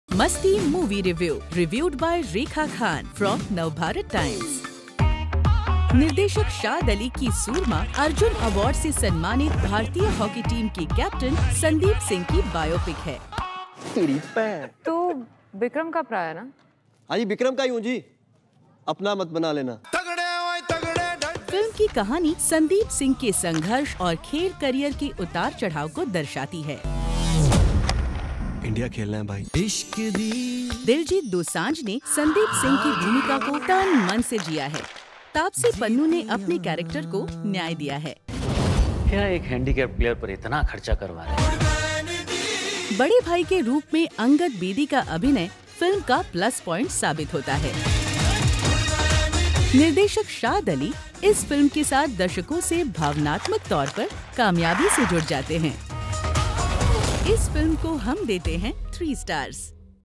Soorma Movie Review | Diljit Dosanjh & Taapsee Pannu
trend trendingvideo youtube netflix Watch the movie review of Soorma starring Diljit Dosanjh & Taapsee Pannu directed by Shaad Ali only on India's No.1 Music & Youth Channel, Mastiii!!